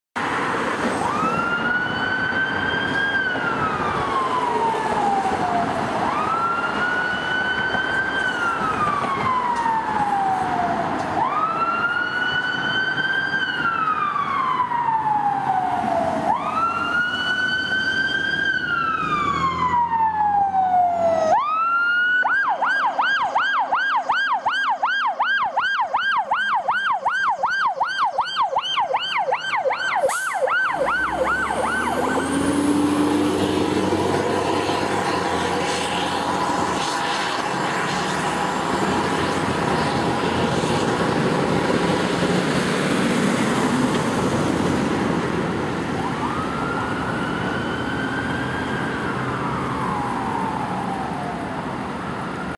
Toronto Fire Aerial 441 Responding
going for Alarm Bells on Islington ave just beside my school